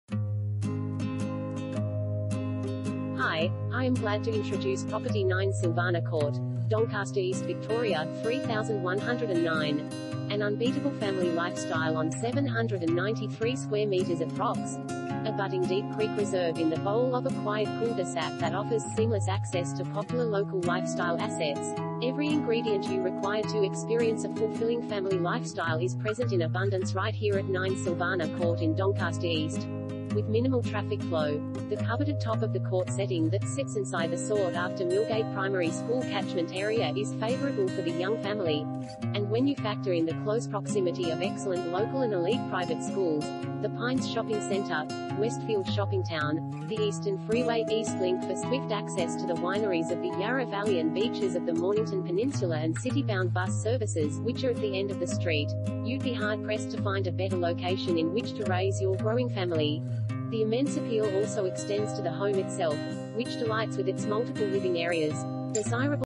9 Silvana Court, Doncaster East VIC 3109 | AuPropertyReport.Com
9 Silvana Court, Doncaster East VIC 3109
Property Type: house
Bedrooms: 3
Bathrooms: 3
Car Space: 2
An unbeatable family lifestyle on 793m2 approx.